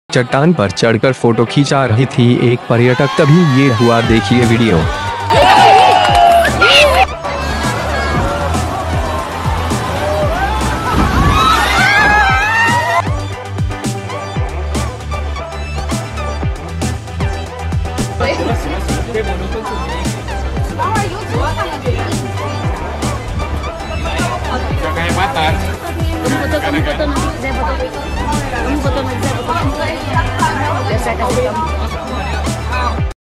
Watch The Terrifying Moment Huge Wave Sweeps Away Tourist Posing On Cliff || Navbharat News
The woman fortunately survived the ordeal with only minor injuries.
A dramatic video has captured the terrifying moment a huge wave swept away a tourist posing on a cliff. Footage currently going viral online shows a woman posing at Devil's Tear on the Indonesian island of Nusa Lembongan, reports Shanghaiist. As she stands with her arms outstretched, a smile on her face, a giant wave knocks her to the ground.
People can be heard screaming in the video as they scramble for safety and the woman disappears from view.
According to Daily Mail, the footage was first shared on Facebook on Friday, and went quickly viral from there.

The unnamed woman fortunately survived the ordeal with only minor injuries. A Bali-based Instagram page shared footage of her being carried by a man and receiving medical attention after the accident. Please stop and be careful! You still have an amazing view 20m from the edge."